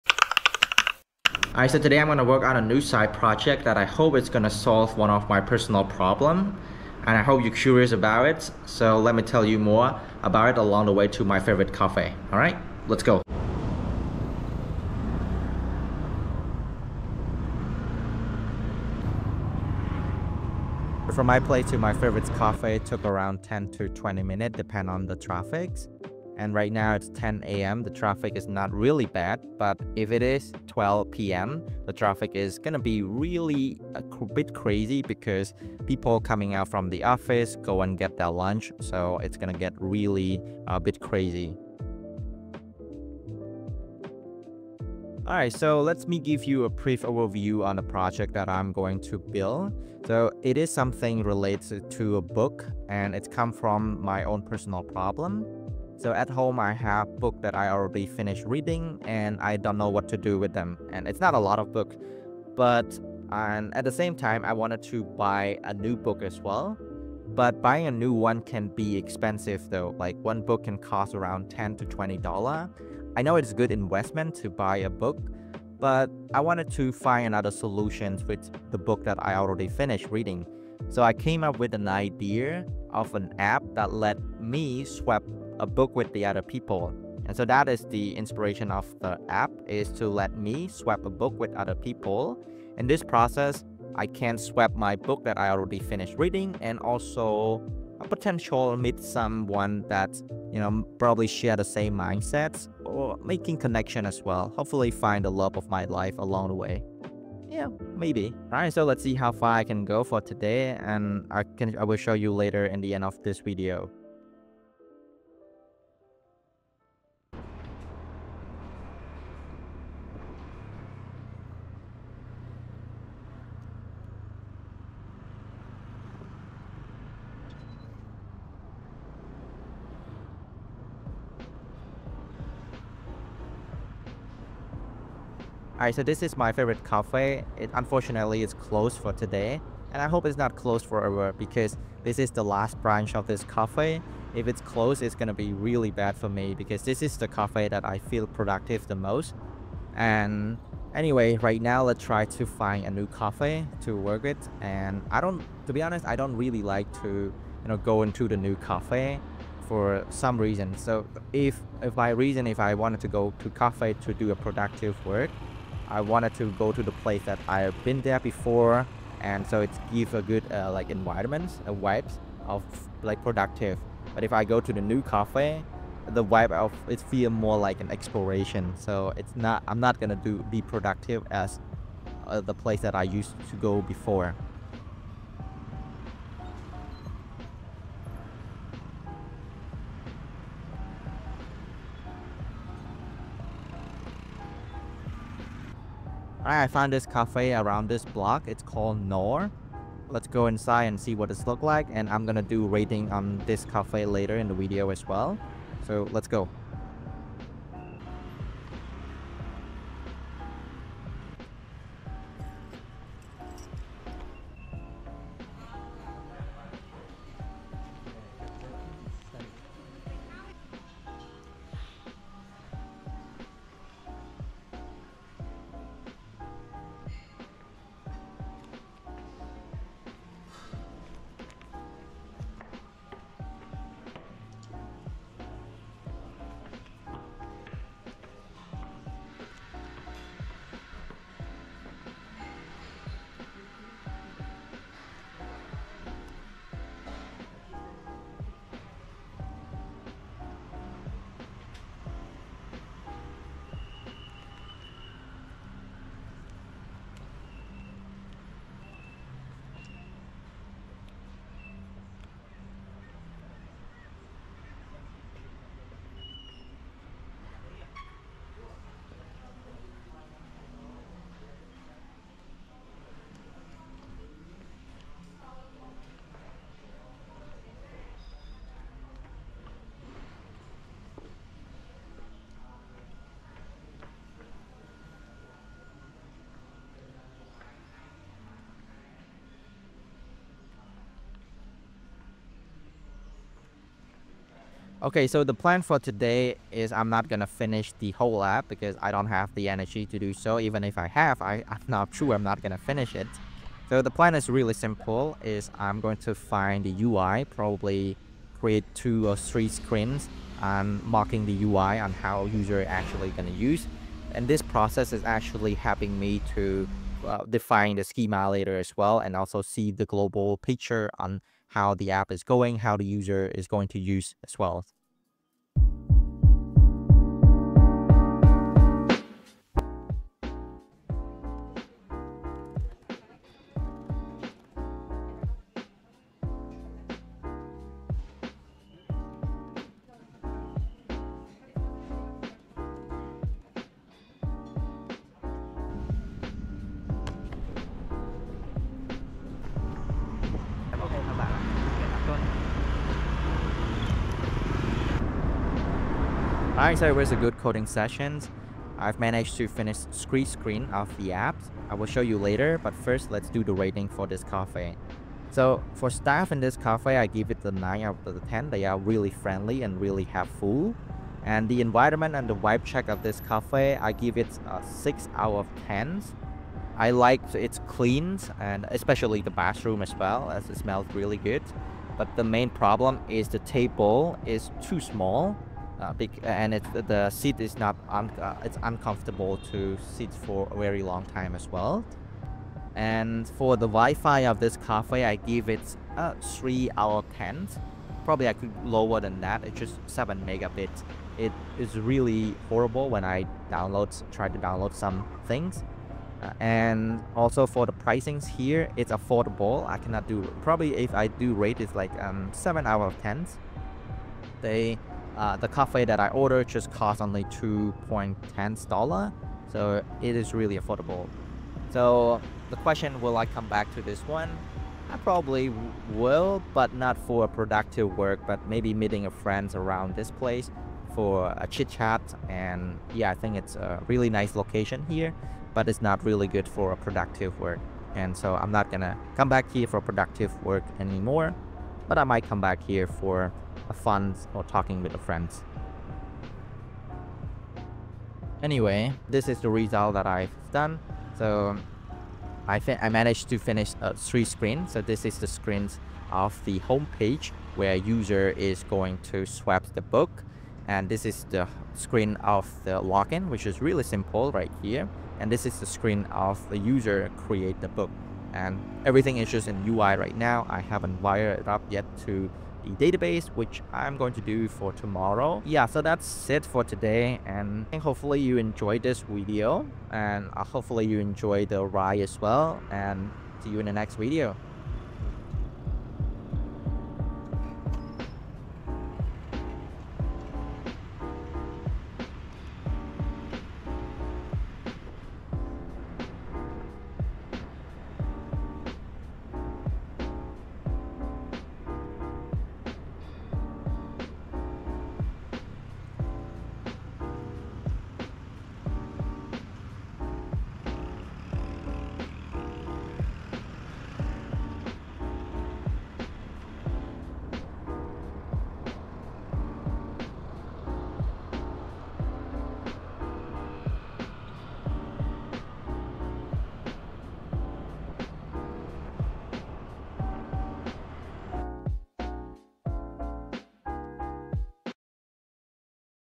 Pov: Coding a New side project to solve my personal problem
In this video I am going to show you how to my favorite cafe and at the same time talk about the project that I am going to build as well.

Scrimba:  Very cool platform to learn code check it out

Music Credit

⭐️ Contents ⭐️

00:00 Intro
00:13 On the way to cafe
00:45 Talk about project
02:16 Explore new Cafe
04:55 Working on the project
06:05 Cafe Rating
07:30 Result of Today
08:05 Enjoy the ride